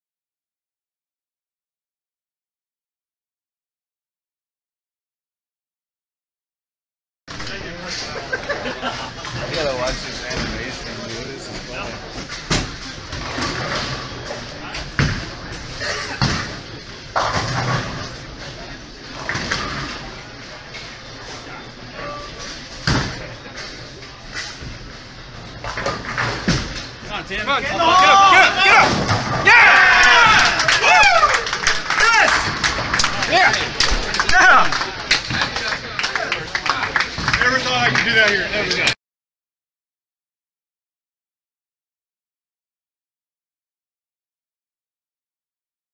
Tim Watanabe 300 Game - Evergreen Lanes North End Masters
Video of the final shot of my 300 game during the North End Masters League at Evergreen Lanes in Everett, WA. It's been 8 long years since my last USBC-sanctioned perfect game (March of 2008 at Kenmore Lanes), in fact I hadn't even had the front 11 since then in league play.

I was pretty nervous, but I trusted all the practice and hard work I've put in and fortunately all the pins responded.

Huge thanks to my awesome teammate Matt Zalupski for recording this and Pat Johns of HiLine Lanes for drilling up an awesome bowling ball!